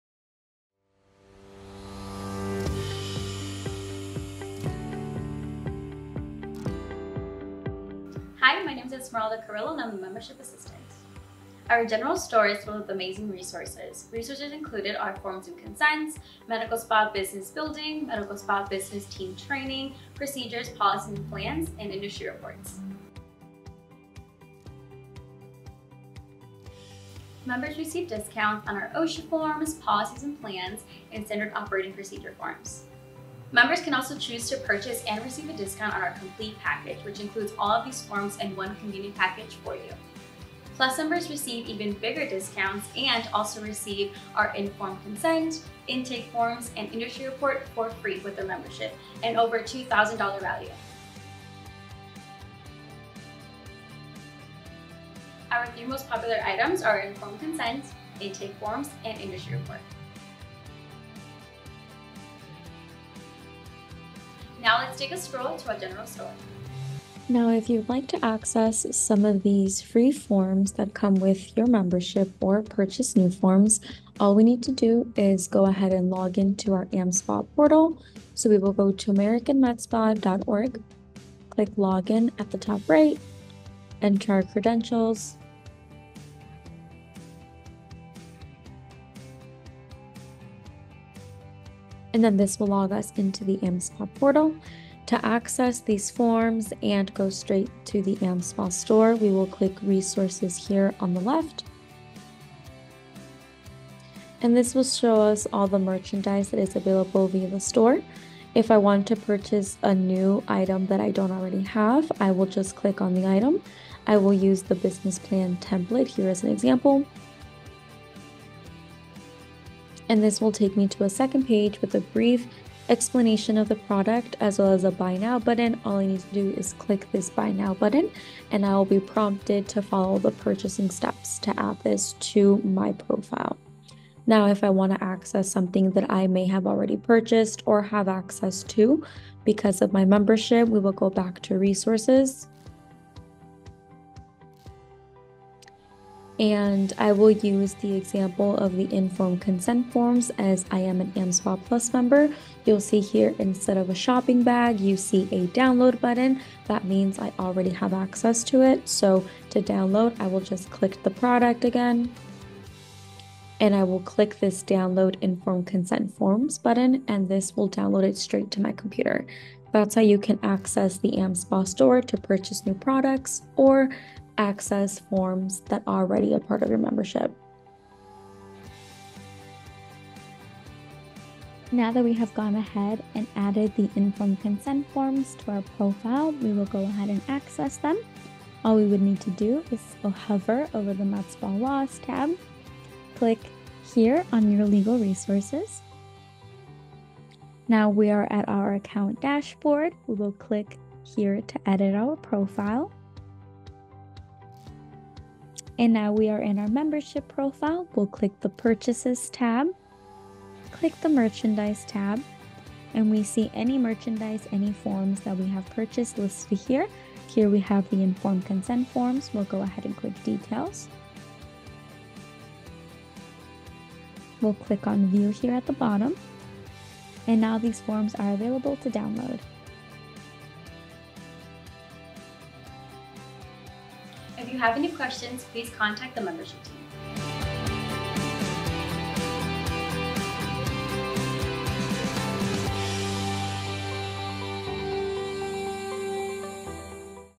AmSpa Membership Academy - Part 7 - General Store (UPDATED)
Follow along as Membership Assistant, Esmeralda Carrillo, walks you through the AmSpa General Store for part 7 of our webinar series.

Timecodes:
0:00 - Intro
0:11 - AmSpa General Store
0:23 - Membership Discounts
0:51 - Most Popular Items
1:01 - General Store Walkthrough
2:58 - Sample Informed Consent and Intake Forms
3:54 - Outro/Contact Us